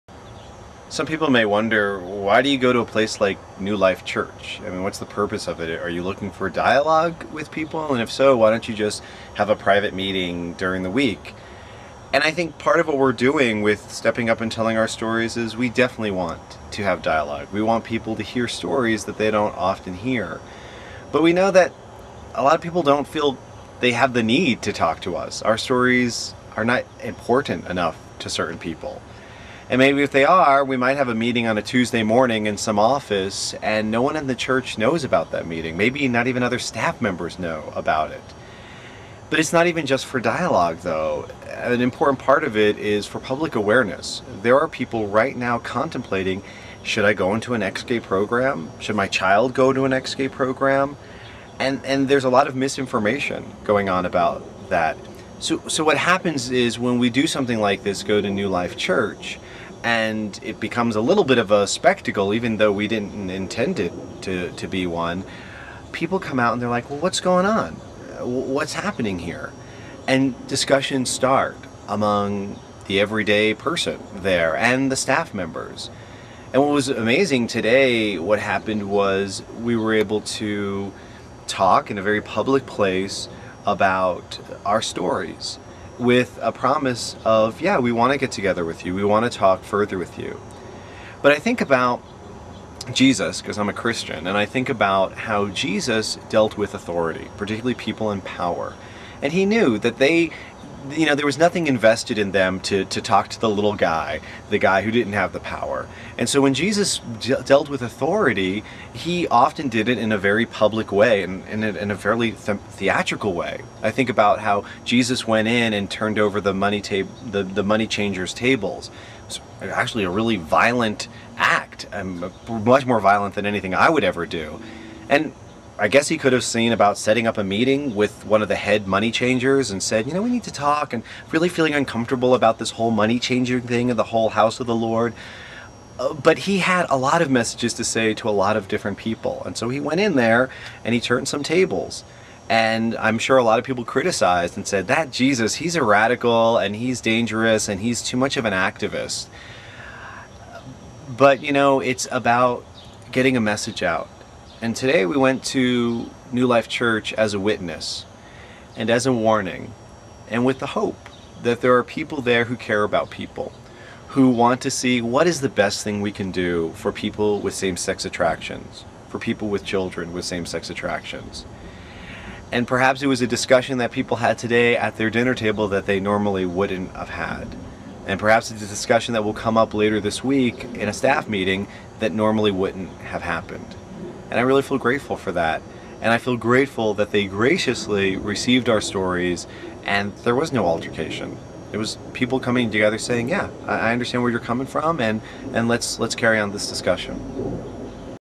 Survivor's Initiative Explained
Ex-gay survivor Peterson Toscano explains why the Survivor's Initiative action at New Life Church in Colorado Springs was meant to be a surprise to church leadership.